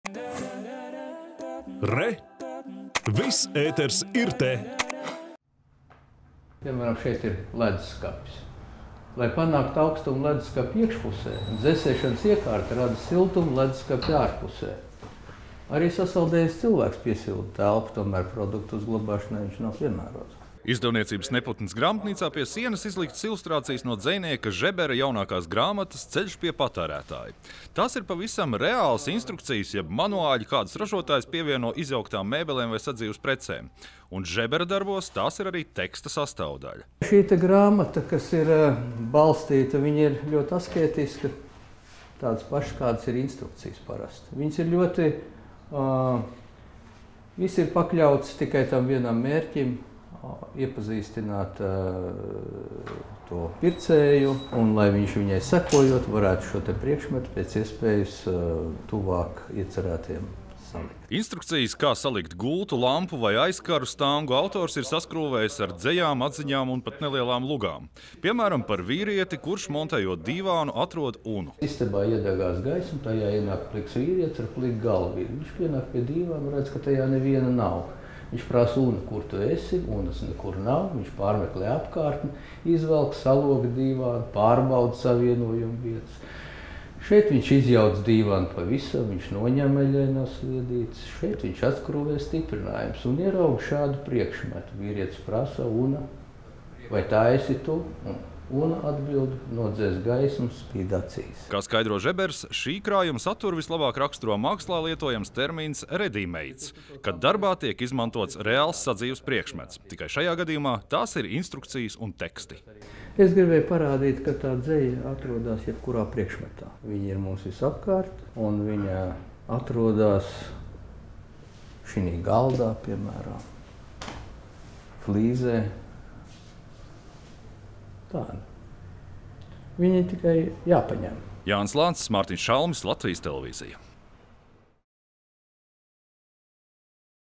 Žebera “Ceļš pie patērētāja”
Lielākai daļai no mums ir nācies iegādāties mēbeles, kuras pēc tam jāsaskrūvē pašam. Un parasti šādam pirkumam līdzi nāk instrukcija, kā pareizi to izdarīt. Dzejniekam Žeberam šādi iespieddarbi ir kalpojuši par sākumu dzejoļiem un ludziņām. Tagad tie ir apkopoti izdevumā ar nosaukumu “Ceļš pie patērētāja”.